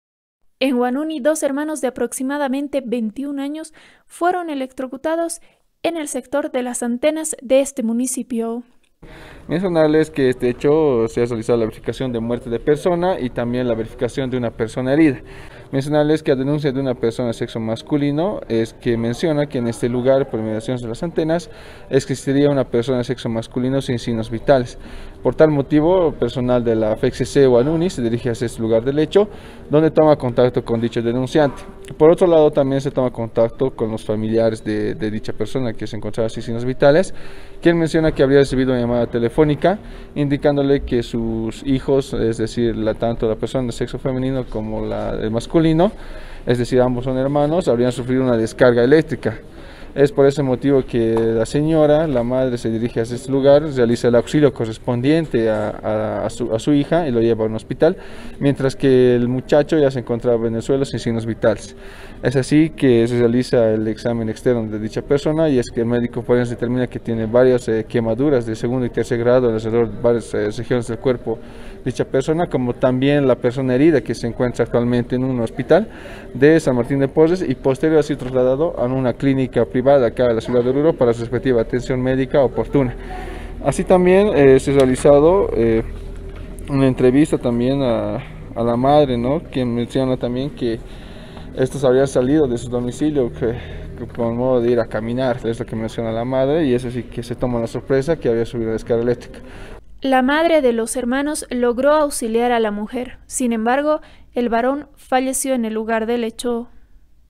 2 HERMANOS ELECTROCUTADOS
En el municipio de Huanuni por el sector de las antenas dos hermanos de aproximadamente 21 años se electrocutaron.